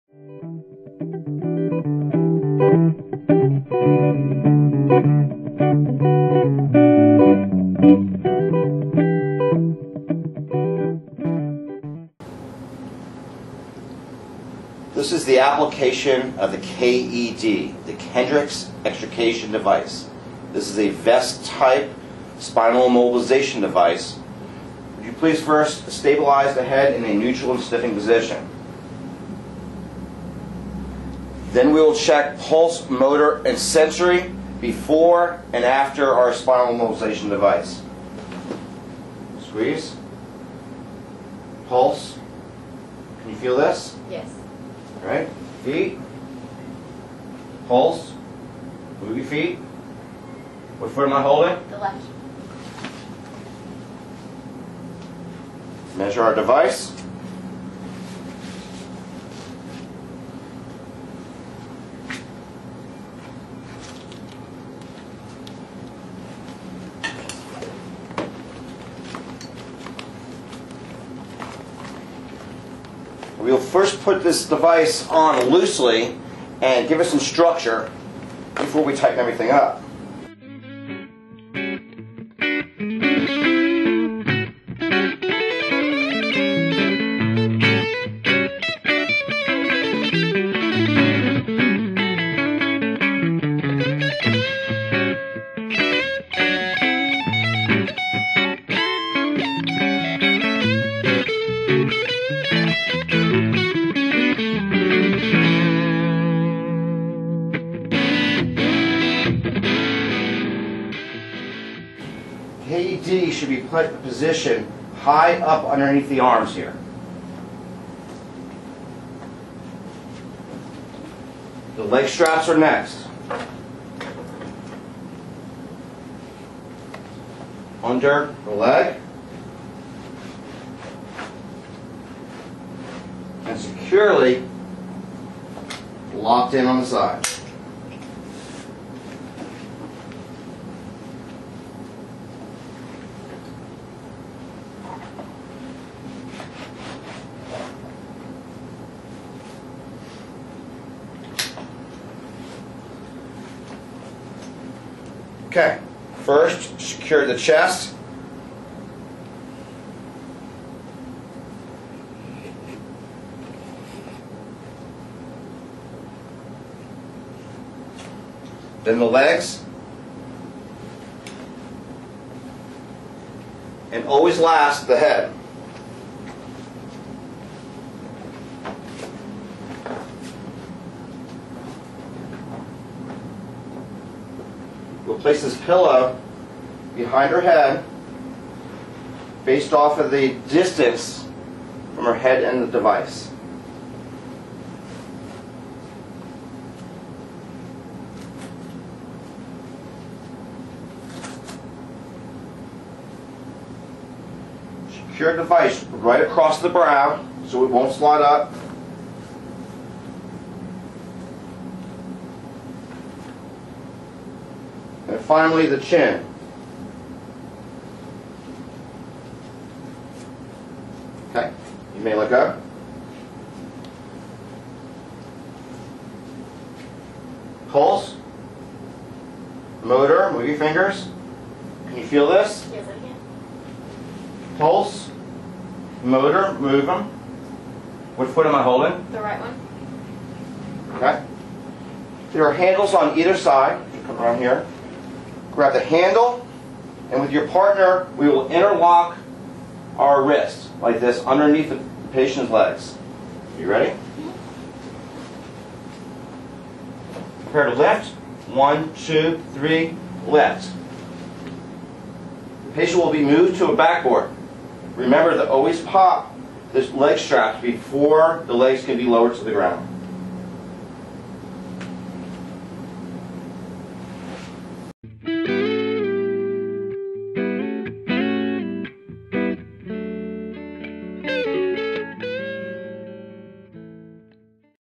Kendrick Extrication Device (KED)
Spinal Immobilization: How to use the Kendrick Extrication Device (KED).